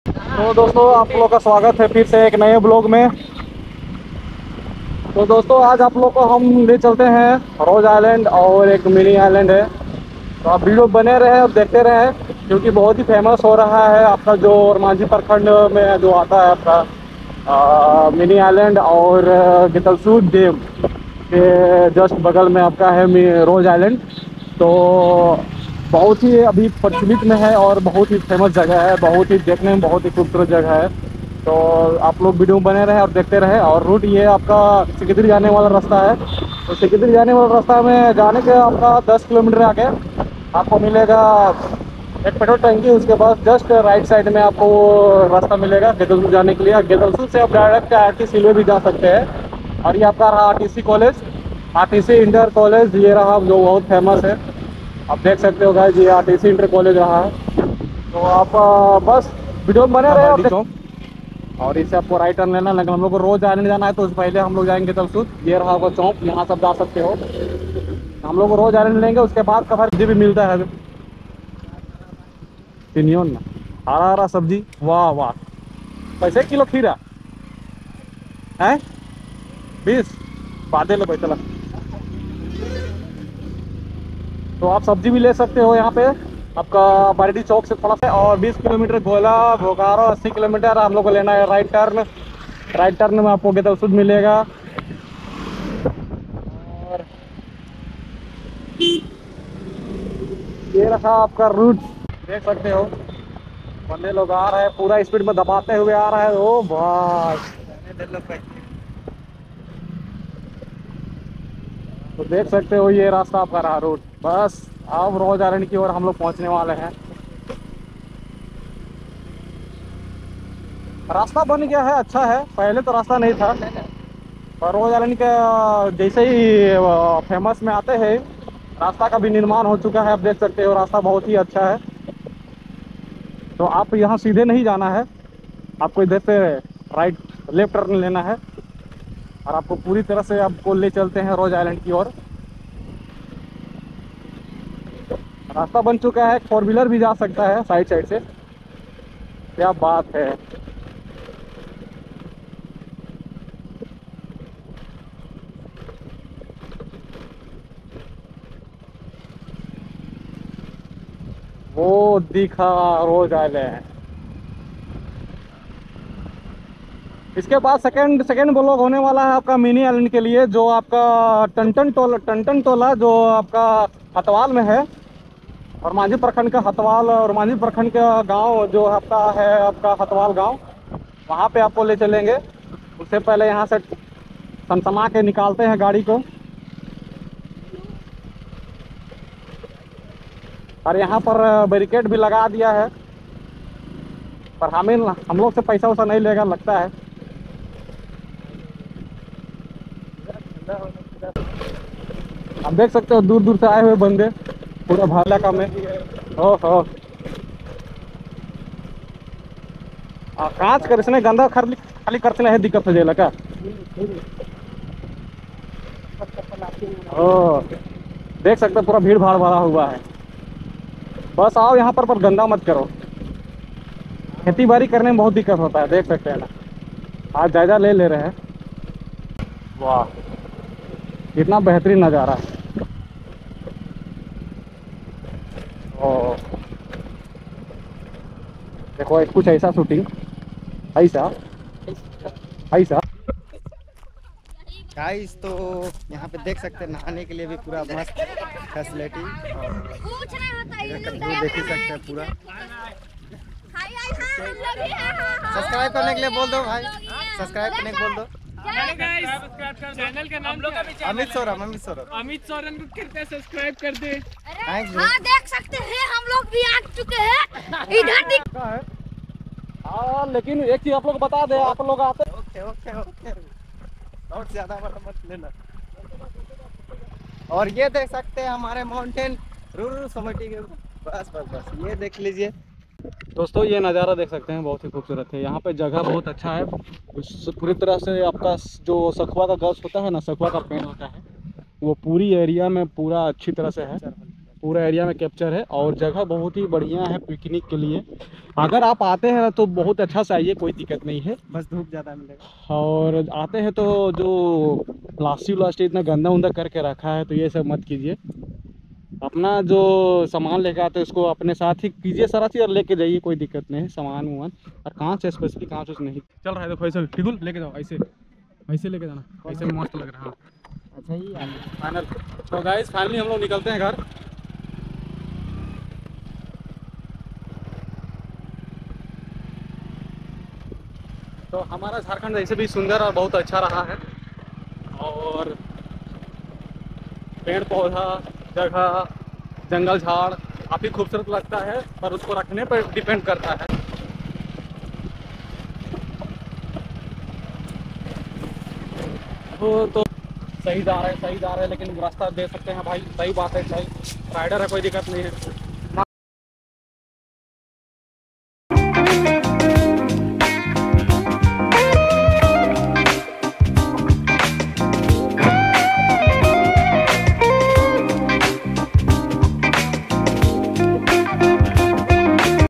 Rose Island 🏝️ | Rose Island Ranchi | Hidden Place Ranchi | Trending Place
rose Island bahut hi acha aur Sundar dikhne wla jagah h aur safty ke mamle me aur acha hai jisme aap picnic bna sakte hai jo gatalsud dam me hai sath hi sath dam ka annad bhi le sakte hai